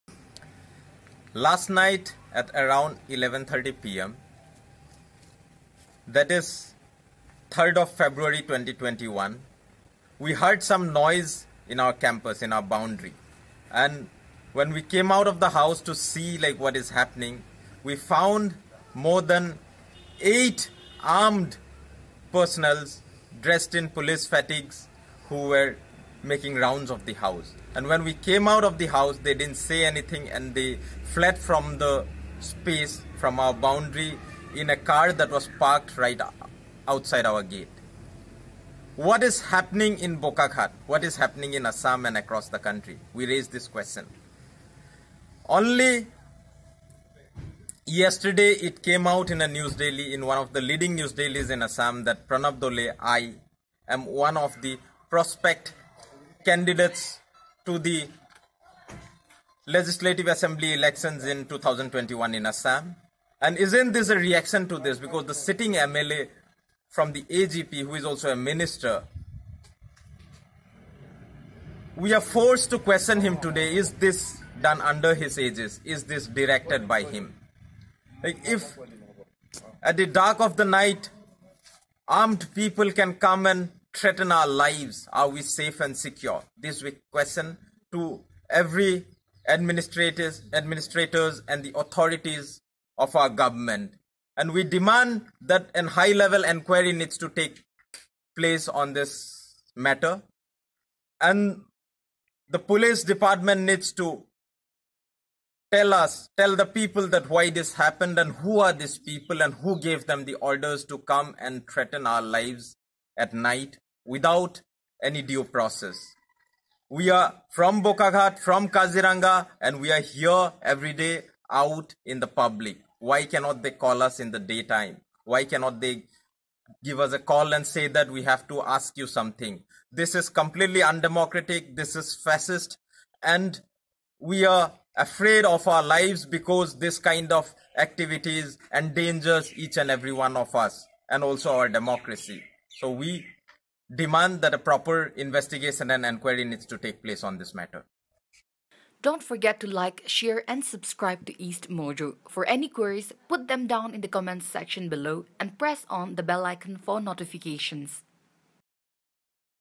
Armed cops barge inside tribal leader Pranab Doley's house in Bokakhat
As Golaghat police denies any involvement, men in police uniform try entering the house of Pranab Doley, a young Mising leader in the Assam poll fray